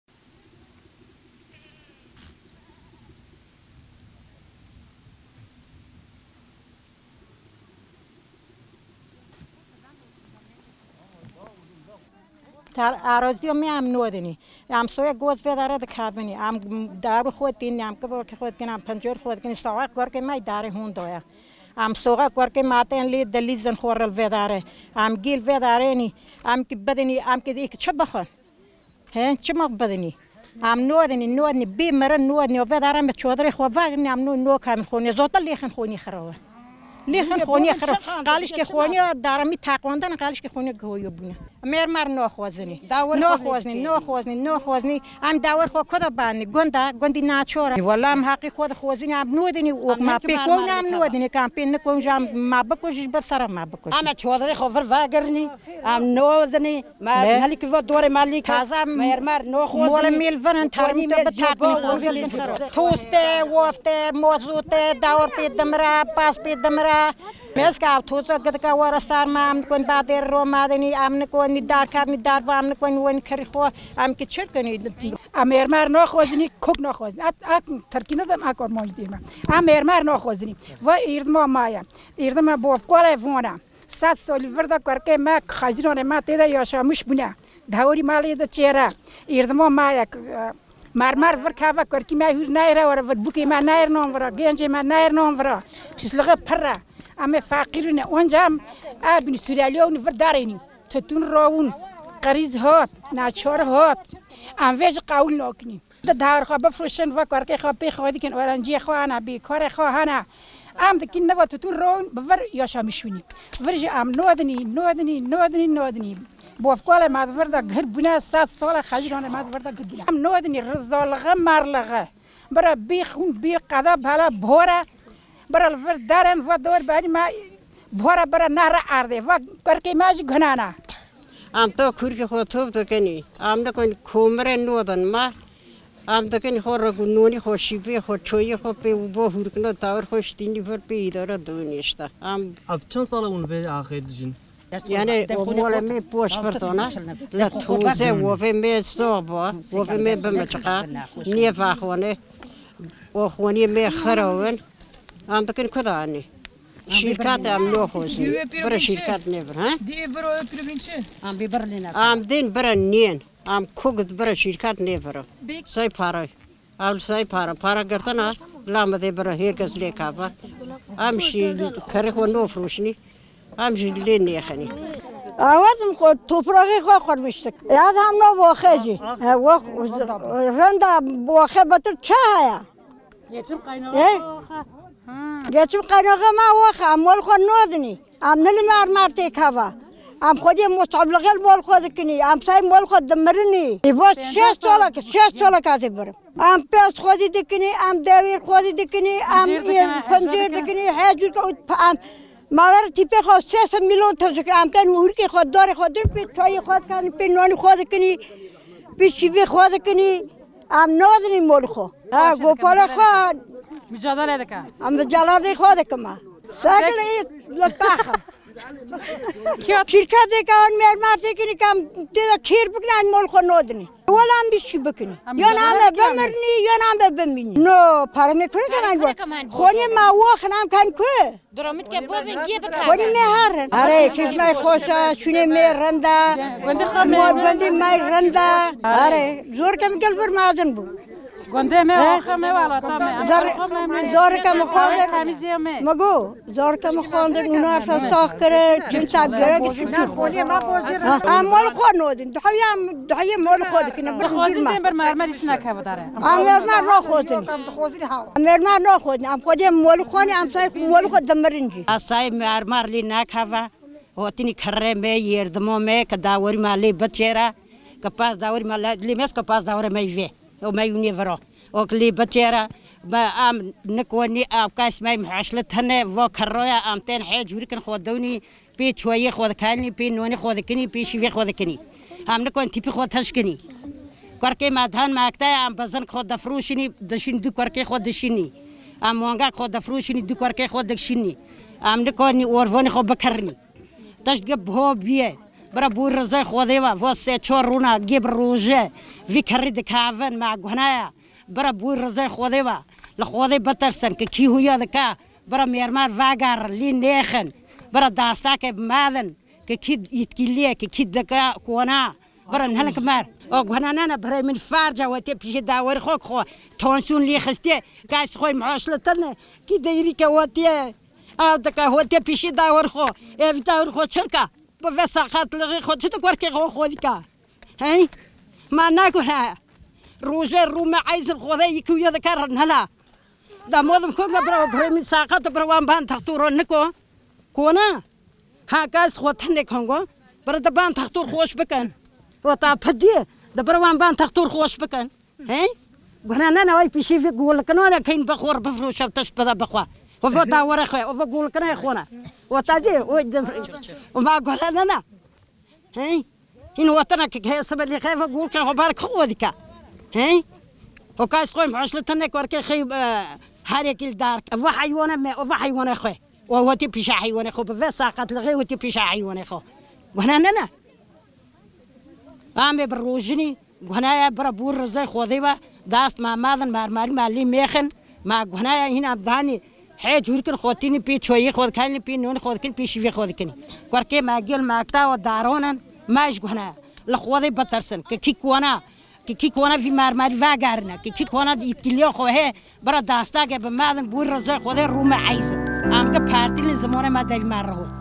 JI BO XWEZAYA XWE JI 7 SALÎ HETA 70 SALÎ TÊDIKOŞIN
Jinên ku di 70 saliya xwe de li hemberî kana mermerê li ber xwe didin diyar kirin ku ax ji bo wa pîroz e û wiha gotin: “Ev der axa me ye. Pêşeroja zarokên me ye. Tûtin ji destê me derxistin lê em xwezaya xwe nadin.”

 Kömür beldesinde mermer ocağına karşı bastonuyla direnen 72 yaşındaki Aşhan Aslan ve beraberindeki kadınlar, “Bastonlarımızla onları durduracağız. Doğamız satılık değil” diyerek kararlılıklarını yineledi.